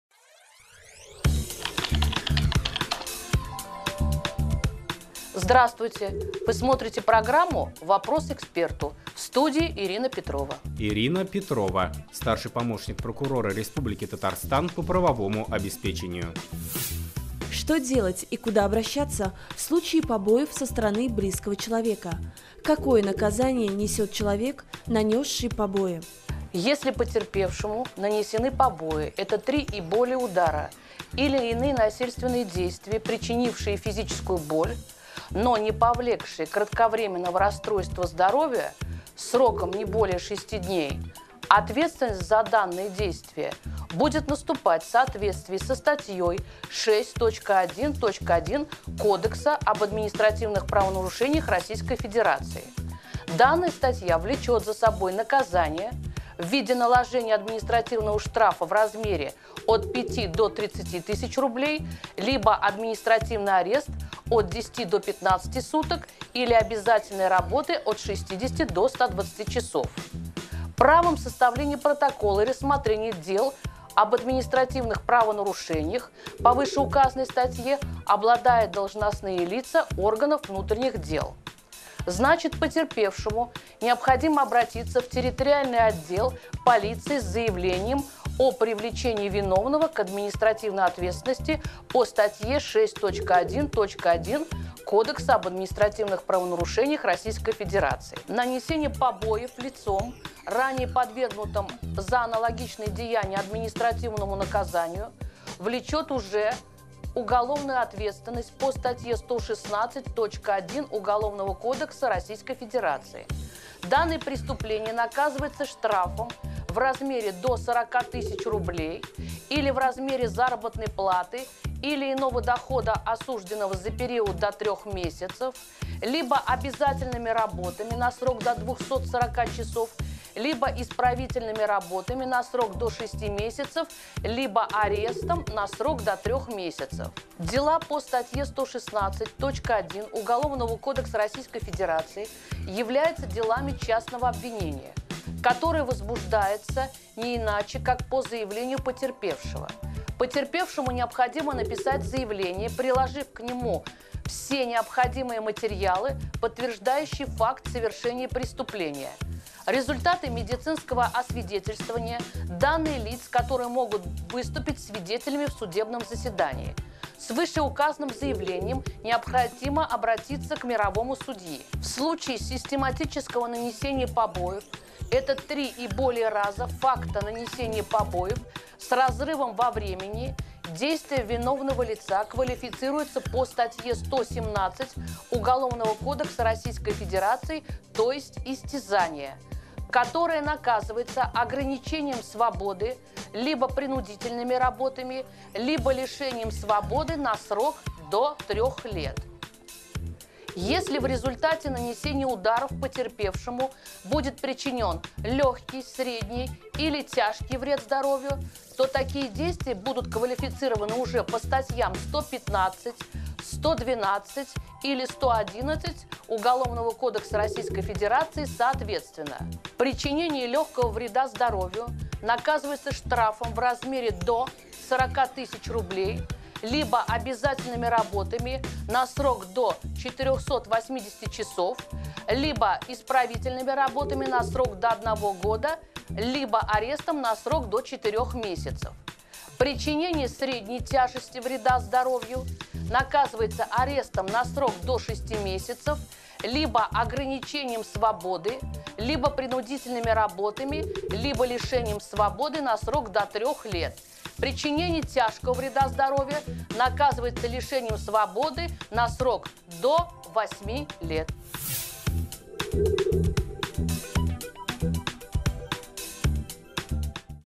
Вопрос эксперту - Ирина Петрова (Прокуратура РТ)
Что делать и куда обращаться в случае побоев со стороны близкого человека? Какое наказание несёт человек, нанесший побои?